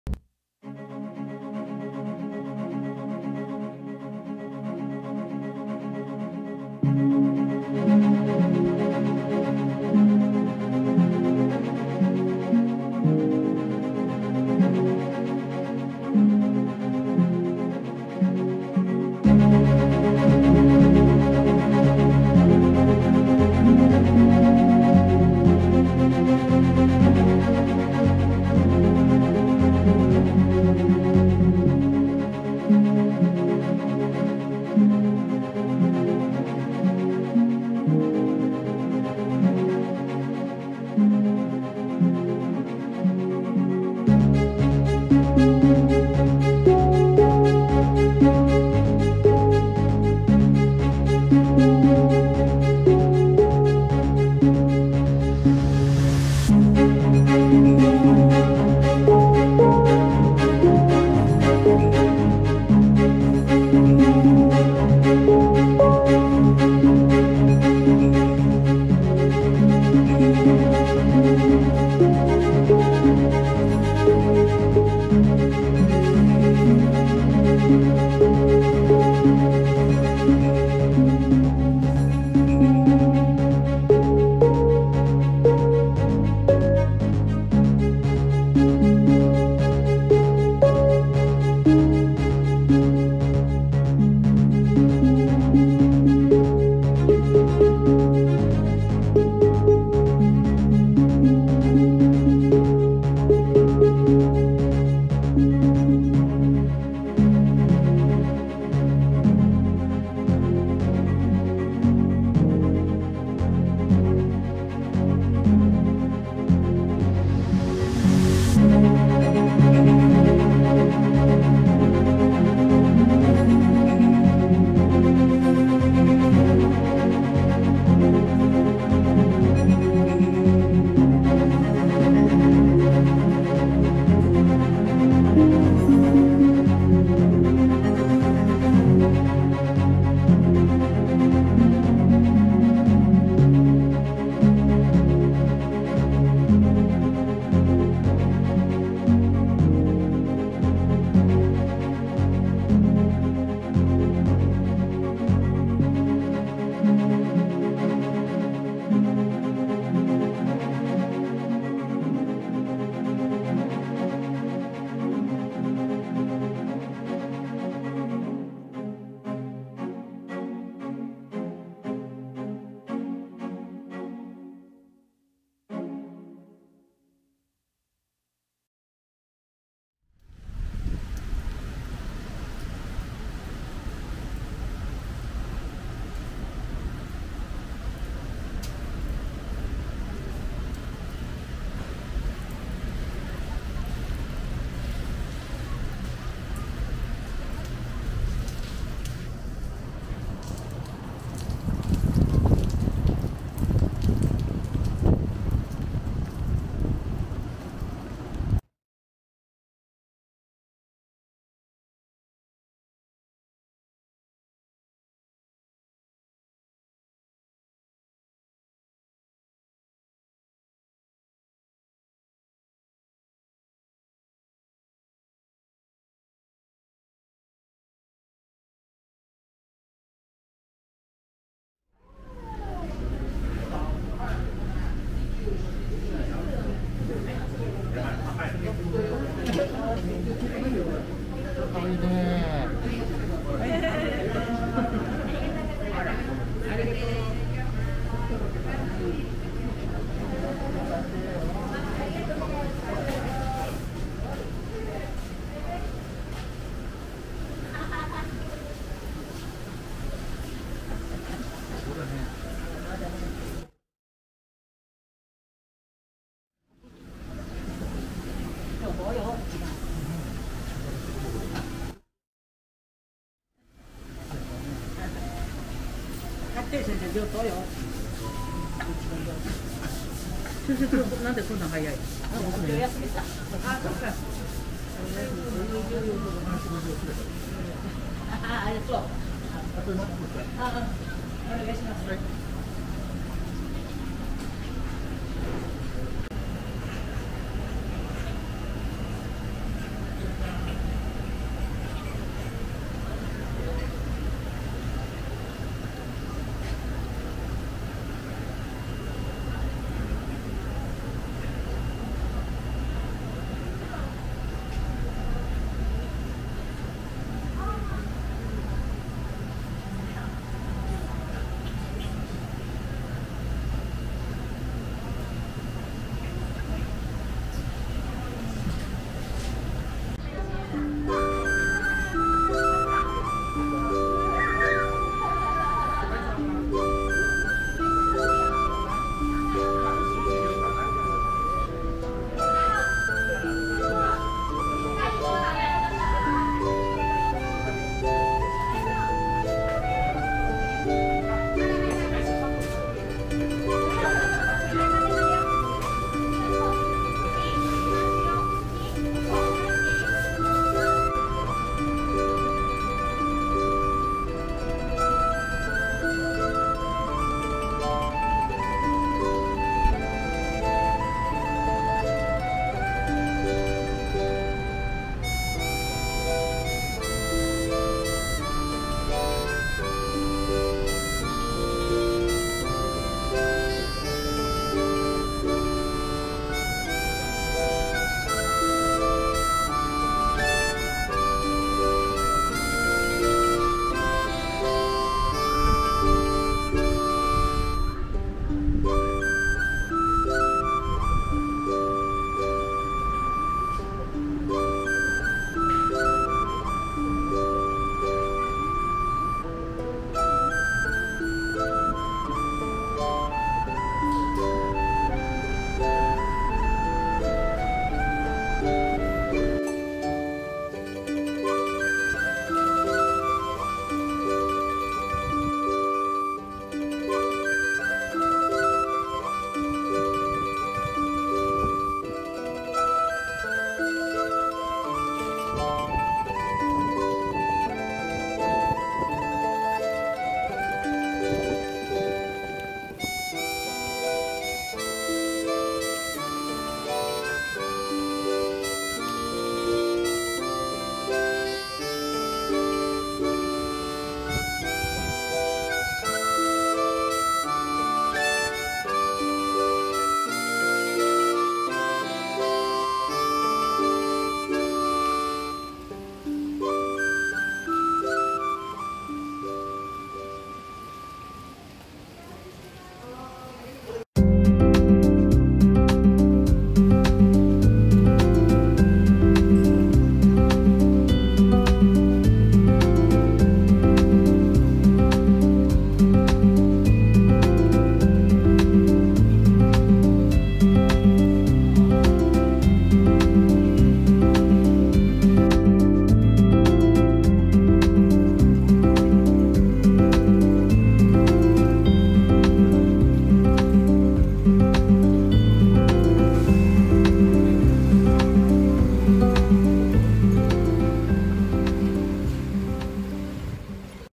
🐱【国際通り・市場周辺：台風6号と緊急事態宣言下の7月24日、4連休3日目】沖縄旅行とメディアによる「誤情報注意」の参考に｡
台風に関しては予報が出ていたし、沖縄の台風の特徴は「強い・遅い」。予報通り、4連休は強風に見舞れ、緊急事態宣言と重なっていますが、観光客の姿は予想よりは多いと思います。